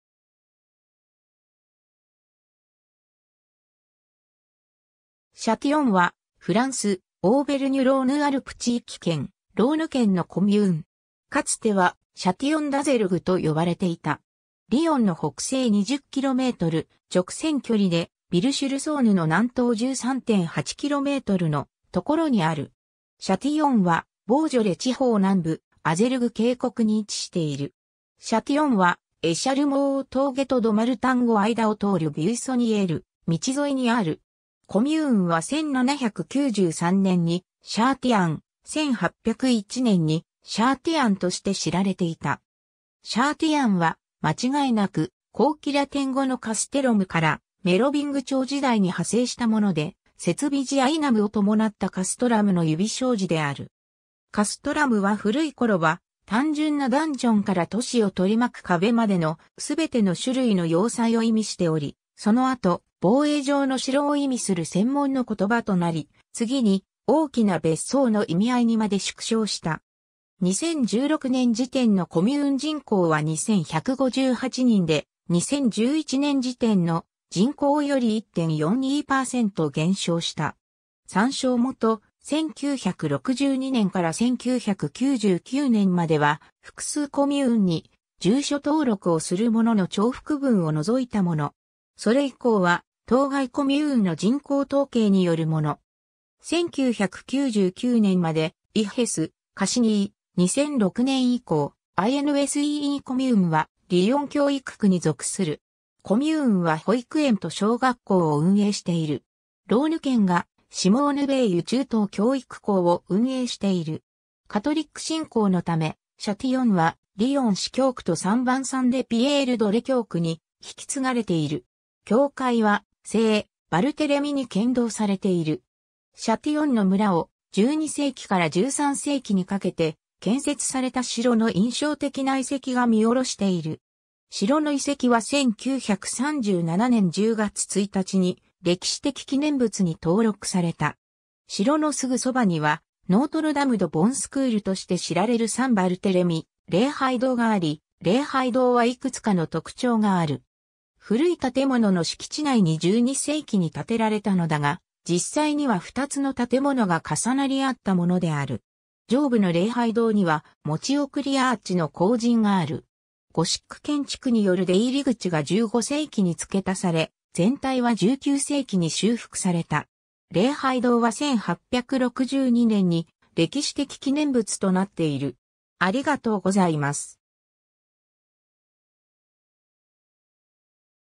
シャティヨン (ローヌ県)
シャティヨン （Châtillon）は、フランス、オーヴェルニュ＝ローヌ＝アルプ地域圏、ローヌ県のコミューン。
かつてはシャティヨン＝ダゼルグ （Châtillon-d'Azergues）と呼ばれていた。
 リヨンの北西20km、直線距離でヴィルフランシュ＝シュル＝ソーヌの南東13.8kmのところにある 。
シャティヨンはボージョレ地方南部、アゼルグ渓谷に位置している。
 シャティヨンは、エシャルモー峠（ソーヌ＝エ＝ロワール県）とドマルタン（ローヌ県）を間を通るビュイソニエール道沿いにある。
 コミューンは1793年にChatillon、1801年にChâtillonとして知られていた。
 Châtillonは間違いなく、後期ラテン語のcastellumからメロヴィング朝時代に派生したもので、接尾辞-ionemを伴ったcastrumの指小辞である。
Castrumは古いころは、単純なダンジョンから都市を取り巻く壁までの、全ての種類の要塞を意味しており、その後『防衛上の城』を意味する専門の言葉となり、次に『大きな別荘』の意味合いにまで縮小した。
 2016年時点のコミューン人口は2158人で、2011年時点の人口より1.42%減少した。
1999年までEHESS/Cassini、2006年以降INSEE コミューンはリヨン教育区に属する。
コミューンは保育園と小学校を運営している。
ローヌ県がシモーヌ・ヴェイユ中等教育校を運営している。
 カトリック信仰のため、シャティヨンはリヨン司教区とサン・ヴァンサン・デ・ピエール・ドレ教区に引き継がれている。
教会は聖バルテレミに献堂されている。
 シャティヨンの村を、12世紀から13世紀にかけて建設された城の印象的な遺跡が見下ろしている。
城の遺跡は1937年10月1日に歴史的記念物に登録された。
城のすぐそばには、ノートル・ダム・ド・ボン・スクールとして知られるサン・バルテレミ礼拝堂があり、礼拝堂はいくつかの特徴がある。
古い建物の敷地内に12世紀に建てられたのだが、実際には2つの建物が重なり合ったものである。
上部の礼拝堂には持送りアーチの後陣がある。
ゴシック建築による出入り口が15世紀に付け足され、全体は19世紀に修復された。
礼拝堂は1862年に歴史的記念物となっている。